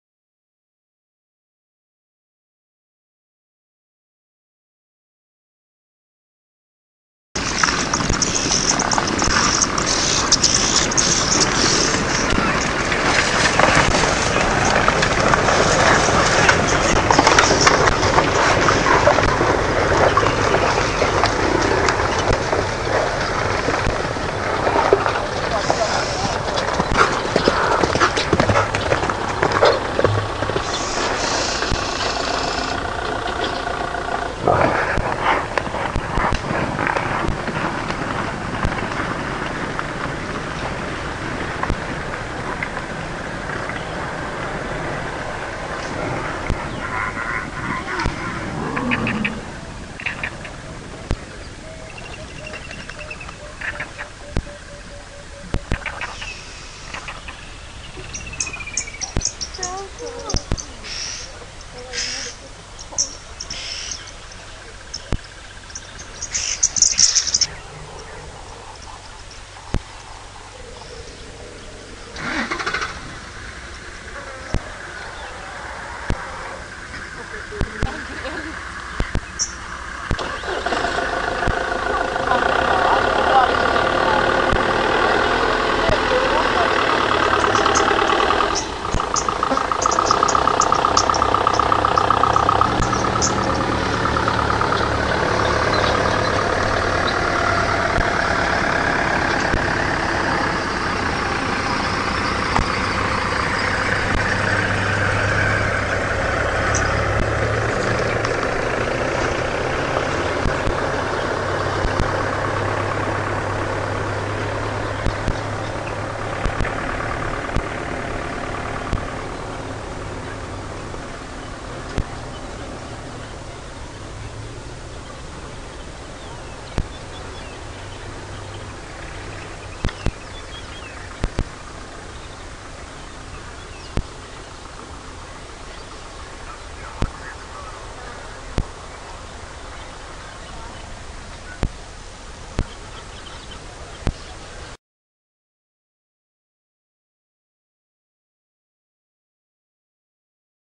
Djuma Game Reserve: Lion's make a charge at the Buffalo after drive Jan 24, 2016
Courtesy of WildEarthTV, Nat.Geo, Djuma Game Reserve and Arathusa Game Lodge